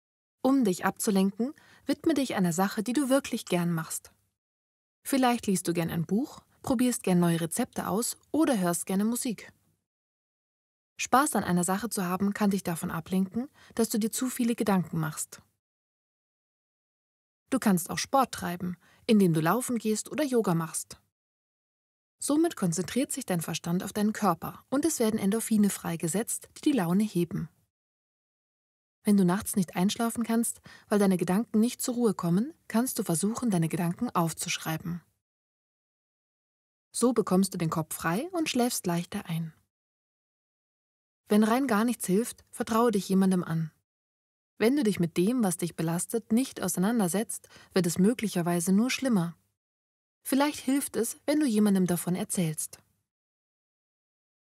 Deine Gedanken Von Etwas Ablenken
Dieses kurze Video gibt dir einige Tipps, wie du Deine Gedanken von etwas ablenken!

Besuche uns in den sozialen Medien und finde weitere hilfreiche und einfache Anleitungen.